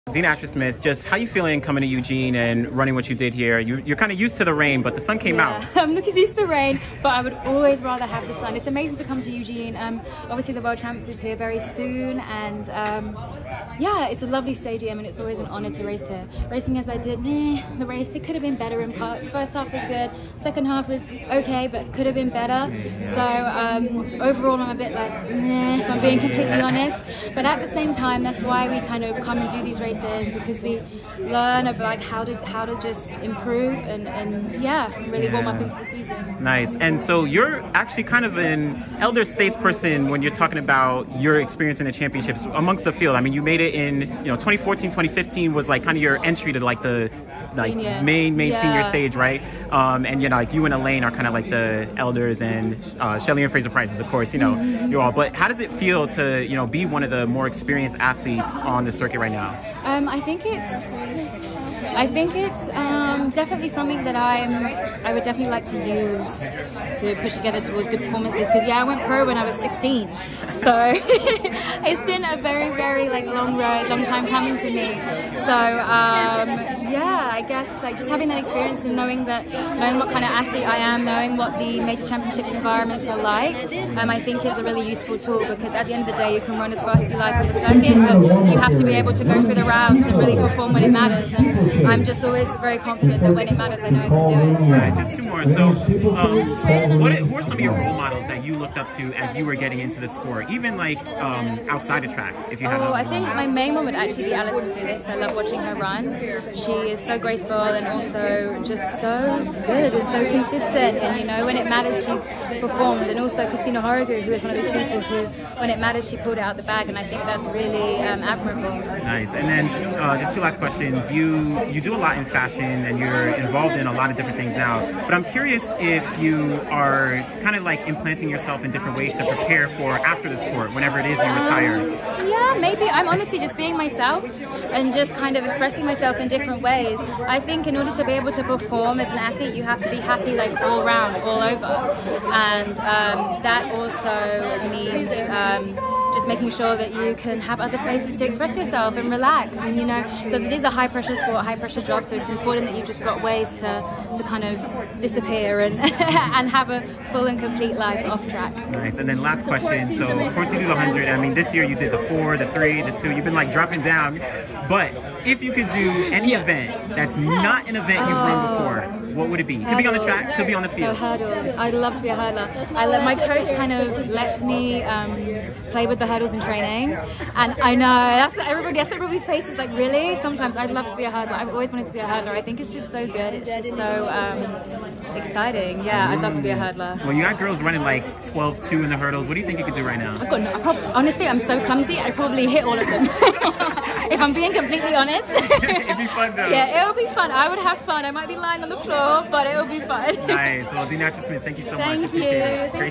Dina Asher-Smith talks Experience, Role Models, and life outside of T&F | 2022 Prefontaine Classic
00:00 - Competing in Eugene
00:36 - Being an Experienced Sprinter
01:36 - Role Models
02:01 - Things DAS is doing outside of track that helps her on the track
02:39 - What other event would DAS do if not sprinting